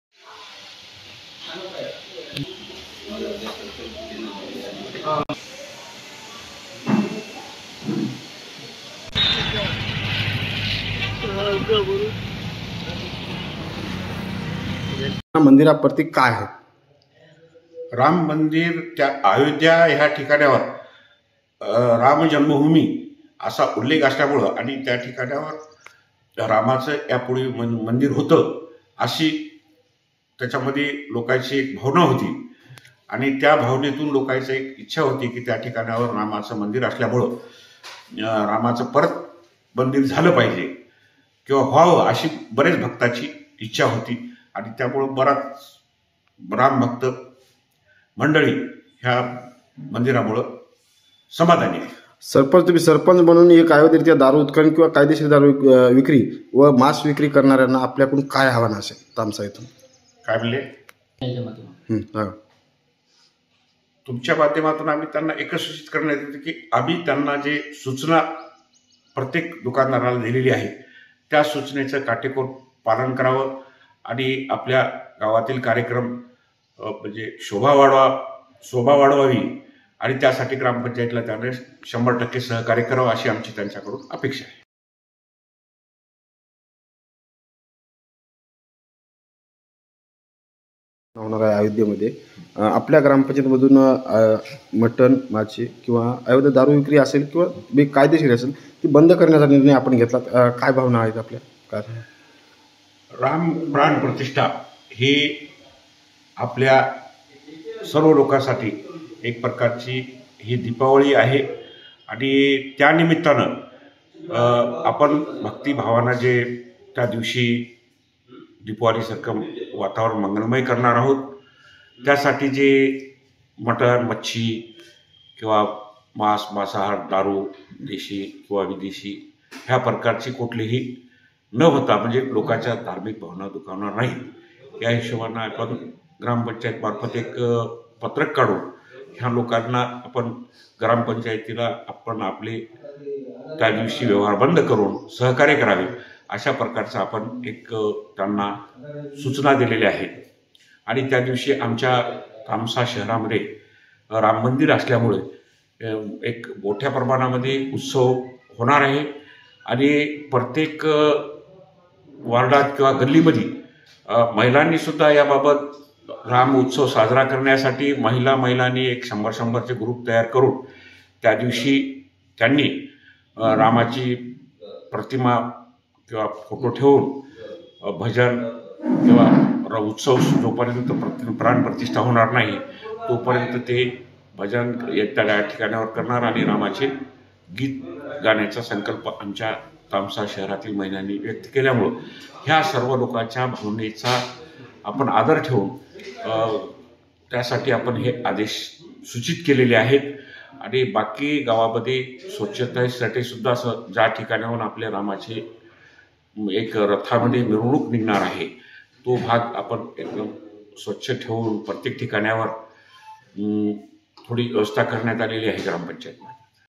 NANDED | राममंदिर लोकार्पण सोहळ्यानिमित्त २२जानेवारीला तासमा येथे विविध कार्यक्रमांचे आयोजन
दिनांक 22 जानेवारी रोजी प्रभू श्रीरामाचा भव्य प्राणप्रतिष्ठापना सोहळा संपन्न होणार आहे. त्याच पार्श्वभूमीवर तामसा येथील ग्रामपंचायत हद्दीतील सर्व दारू विक्रेते व मांस विक्रेत्यांना सूचना दिल्या आहेत की दिनांक 22 जानेवारी रोजी कुठल्याही प्रकारे दारू व मास विक्री करू नये व ग्रामपंचायतला सहकार्य करावे असे ग्रामपंचायत सरपंच आनंदराव घंटलवार यांनी सांगितले आहे. तामसा येथे प्रभू श्रीरामाचे मंदिर असल्यामुळे तिथेही भव्य सांस्कृतिक कार्यक्रमांचे आयोजन करण्यात आले आहे. यावेळी प्रभू श्रीरामाची पालखी सुद्धा निघणार आहे. त्याबरोबर सर्व तामसा गावांमध्ये ग्रामपंचायत स्वच्छता अभियान देखील राबविण्यात येणार आहे.

BYTE- आनंदराव घंटलवार, सरपंच